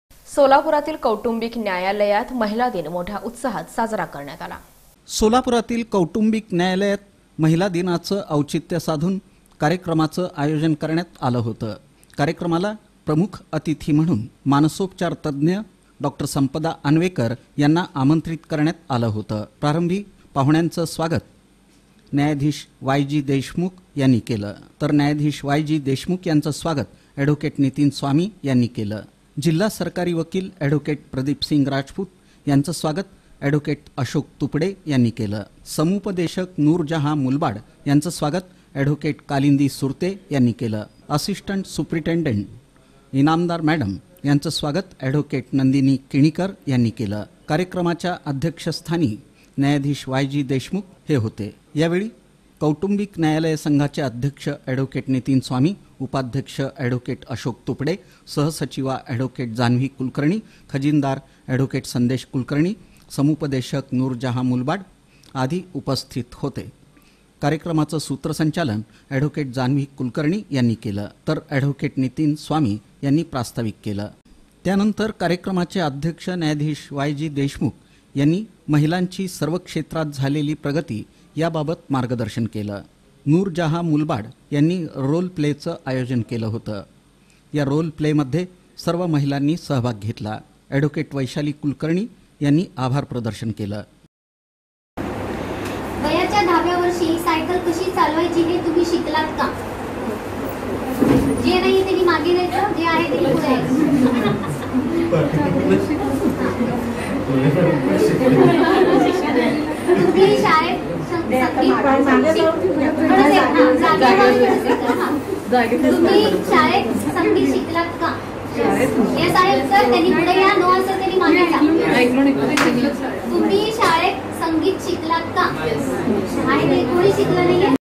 कौटुंबिक न्यायालयाचा उपक्रम... जागतिक महिला दिन उत्साहात साजरा... | KOUTUMBIK NYALYA
कौटुंबिक न्यायालयाचा उपक्रम... जागतिक महिला दिन उत्साहात साजरा...